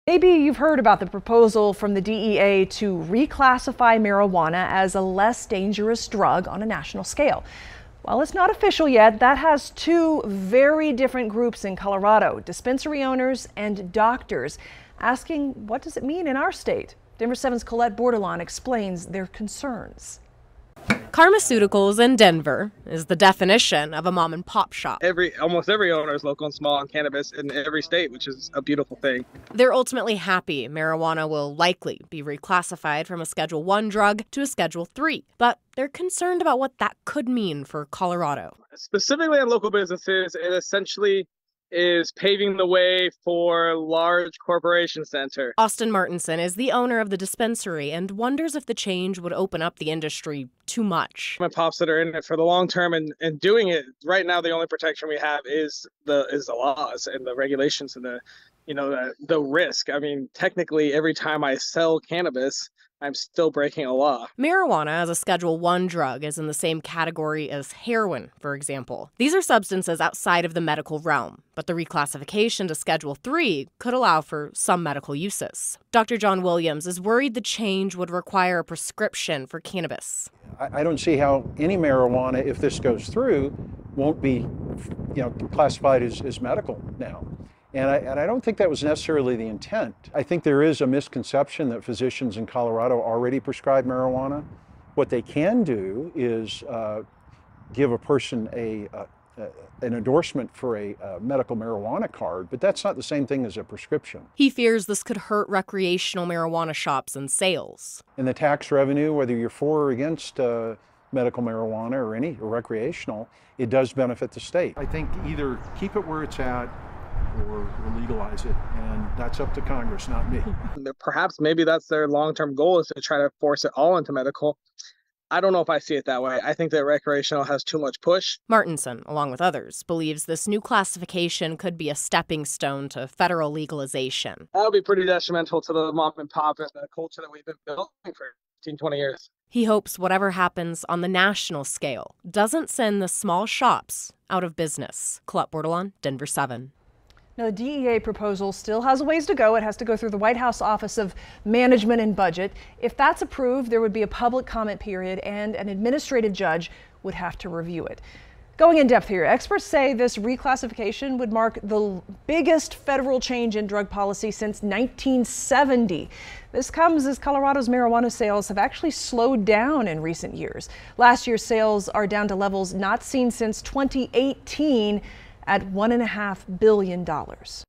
Colorado dispensary and doctor discuss concerns surrounding move to reclassify marijuana
Following The Associated Press report that the DEA will move to reclassify marijuana from a Schedule I drug to a Schedule III drug, certain concerns have arisen in Colorado about what the change could mean.